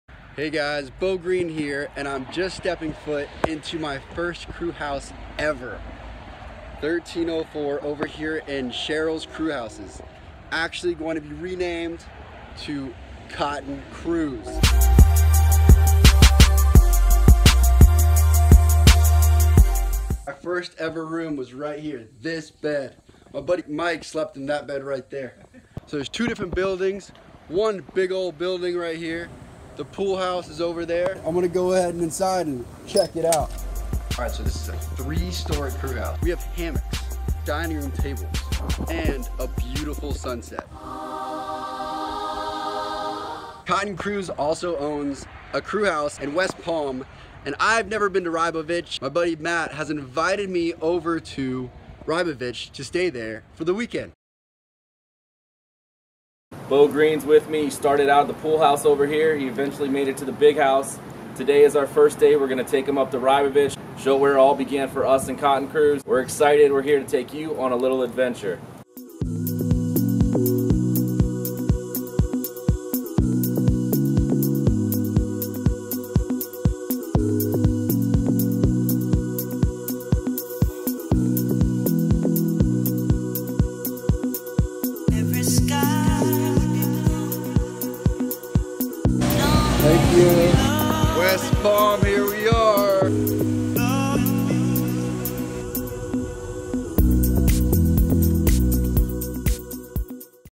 My First Crewhouse (Cotton Crews)
My first brewhouse ever was Cottons. After a few months, I get back to Fort Lauderdale and re- visit my first crewhouse ever. The owner Matt Woodbury shows me what Cotton Crews is all about and invites me to check out his other crewhouse in West Palm